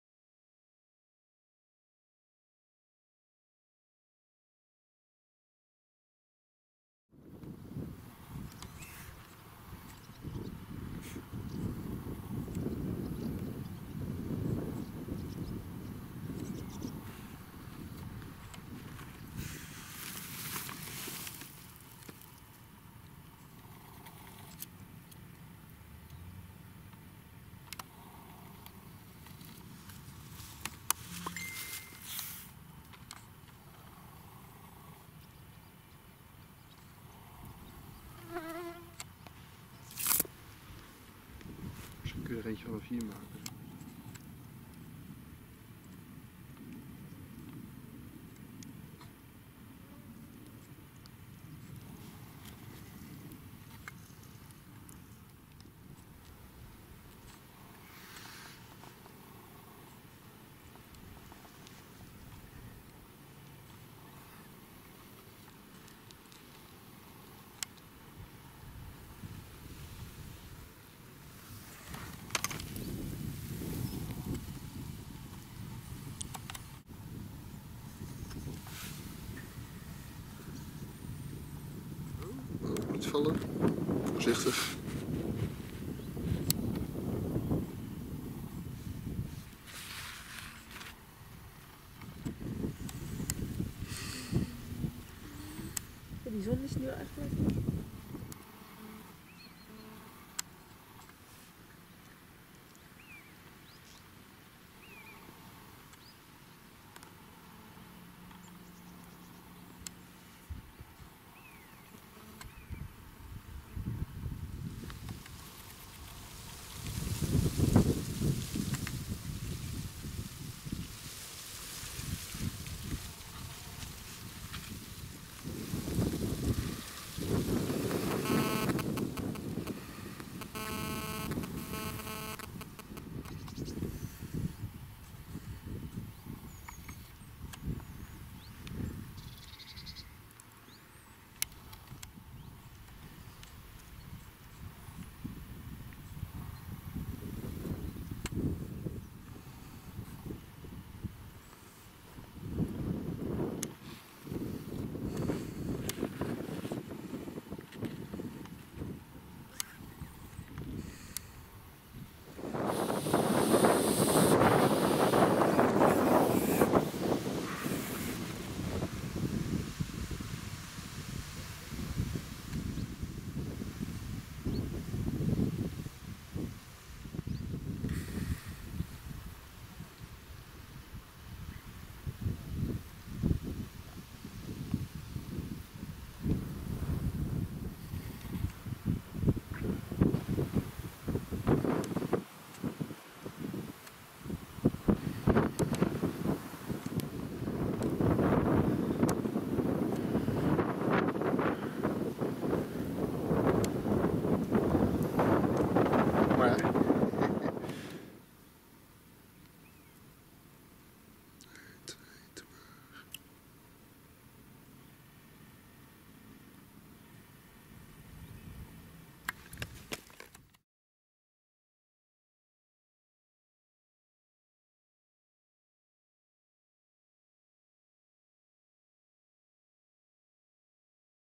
Herping for Adders (Vipera Berus) - Holland (Drenthe) - Herping for Vipers/Snakes
In the late summer of 2015, we went to the moors of Drenthe, a northern province of The Netherlands. We went looking for the adder, or european viper, with a local expert. We found over twenty specimens. Especially the number of juvenils was noteworthy.

Adders op de Drentse heide.